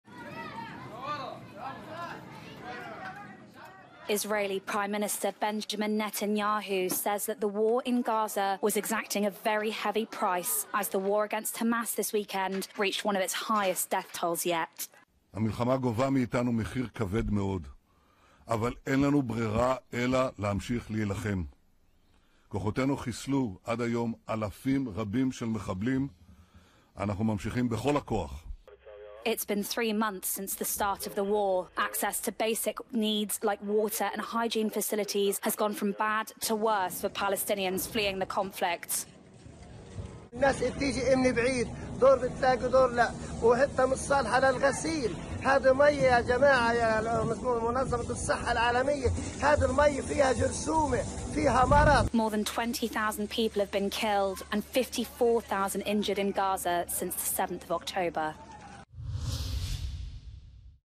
Netanyahu: Israel paying "heavy price" for war
Israeli Prime Minister Benjamin Netanyahu says his side have paid a "heavy price" for the war in Gaza; following one of its deadliest weekends of fighting so far.